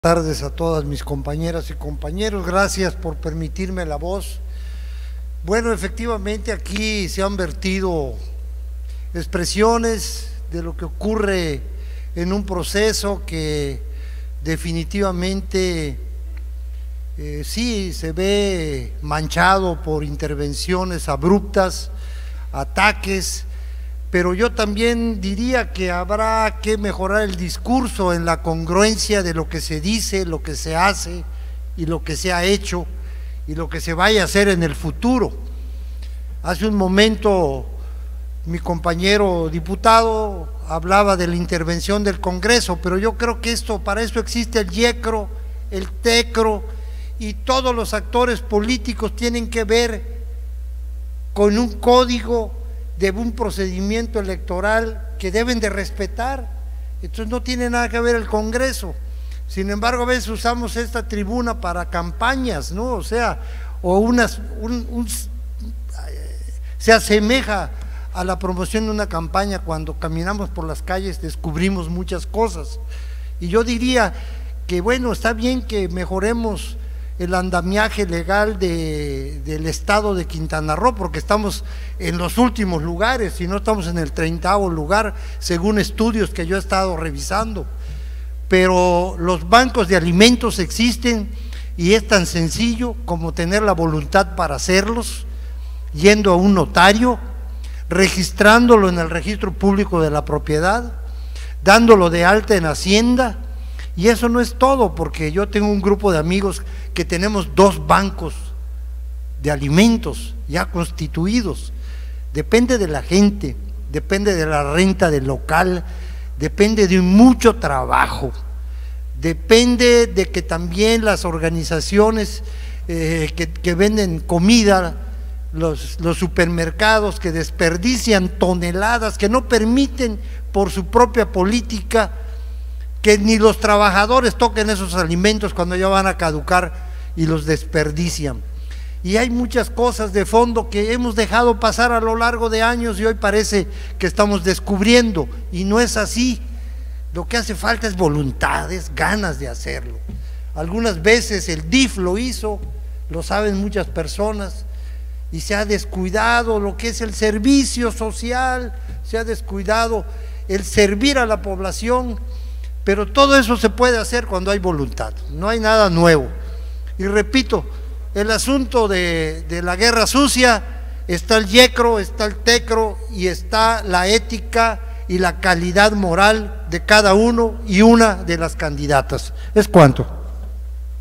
Intervención del Dip. Roberto Erales Jiménez. (26/Abril/2022)
Intervención del Dip. Roberto Erales Jiménez, Presidente de la Comisión Puntos Legislativos y Técnica Parlamentaria de la XVI Legislatura, en la Sesión Ordinaria No. 20 del Segundo Periodo Ordinario de Sesiones del Tercer Año de Ejercicio Constitucional.